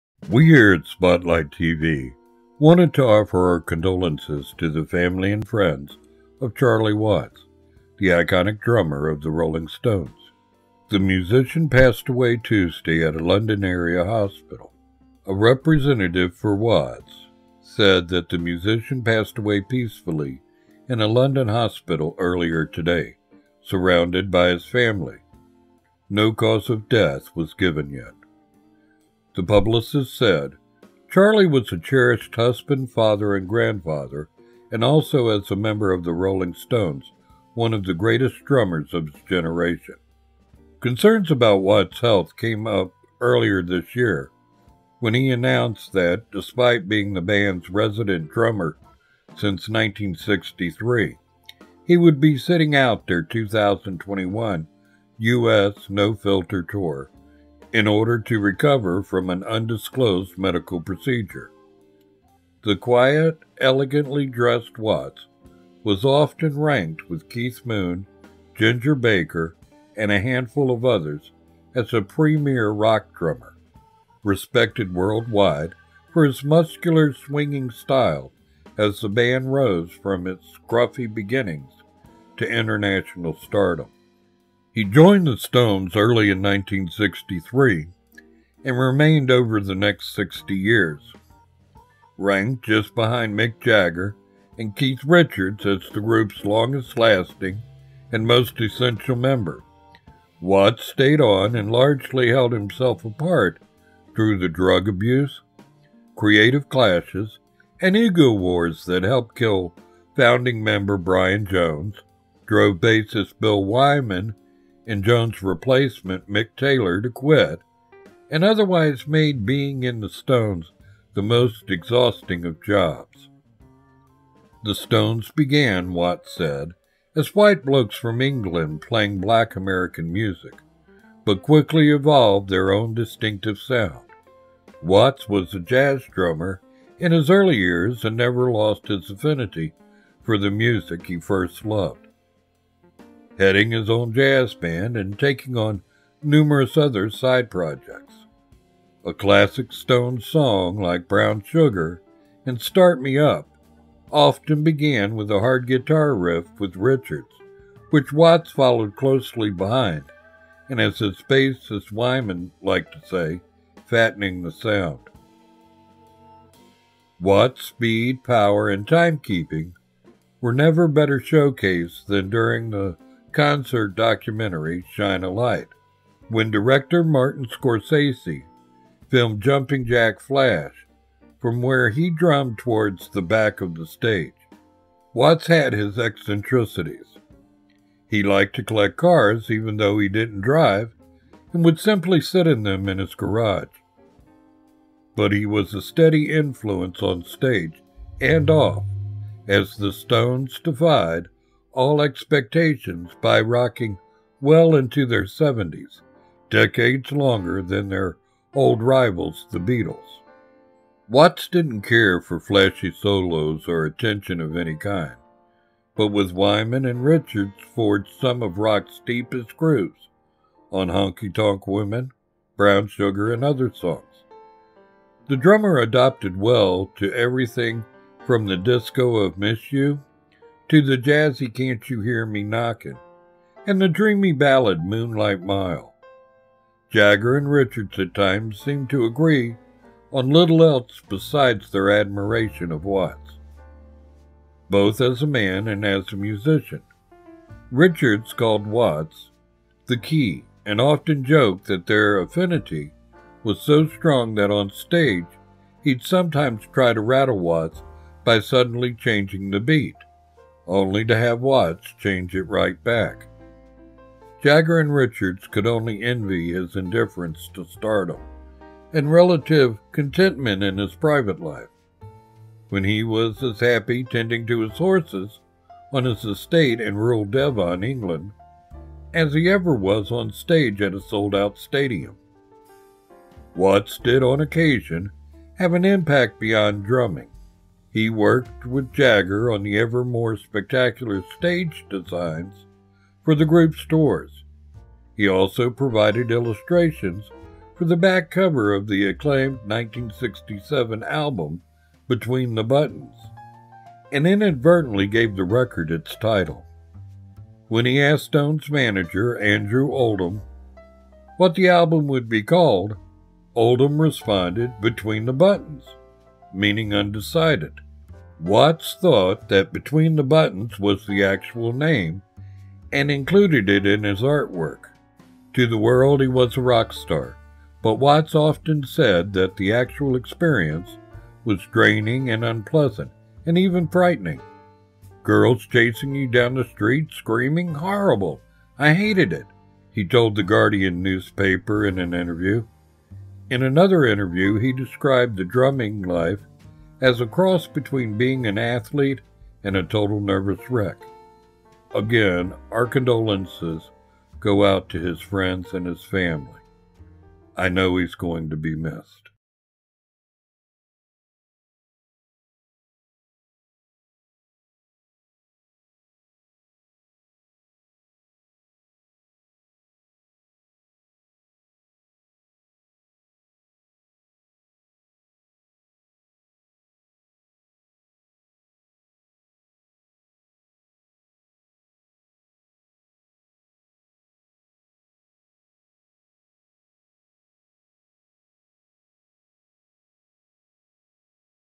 Rest In Peace Charlie Watts
In this video we dim our spotlight for the Iconic Drummer of the Rolling Stones Charlie Watt.